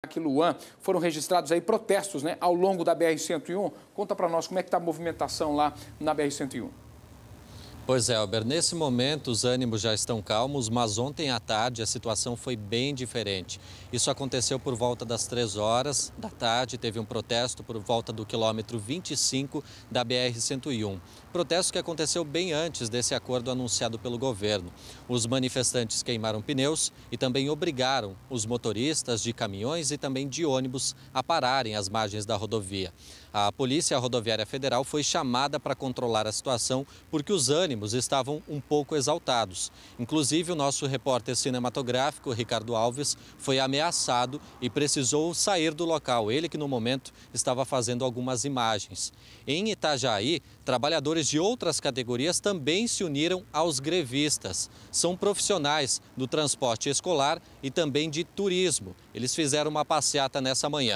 Protestos ao longo da BR-101 para conseguir apoio ao movimento
Protestos ao longo da BR-101. Confira com Luan Vosnhak!